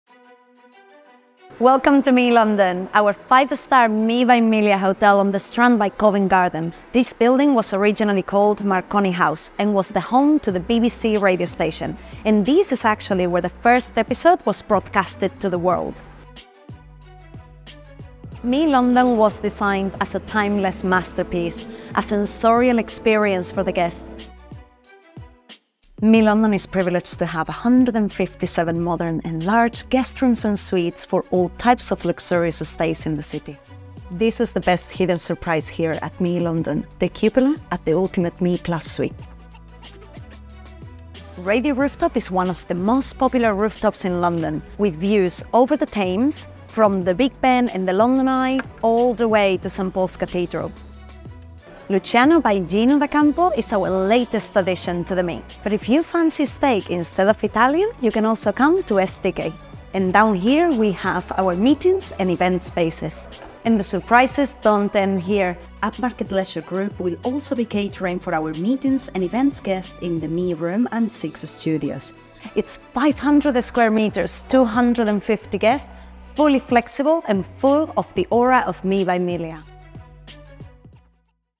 ME LONDON MEETINGS & EVENTS | SITE INSPECTION VISIT
ME London is so much more than a hotel. It’s an architectural masterpiece bursting with art, music, design, and fine cuisine. Revisiting the history of this iconic building, we celebrate its 10th anniversary big time, ME London style. Here's to 10 more years of creativity and innovation in London's cultural epicentre.

At ME London, we work hard every day to create unique experiences with their own personality. Daring events that stand out. They speak for themselves and are a real expression of distinction. Book your next event with us and enjoy modern facilities and exclusive, tailored services.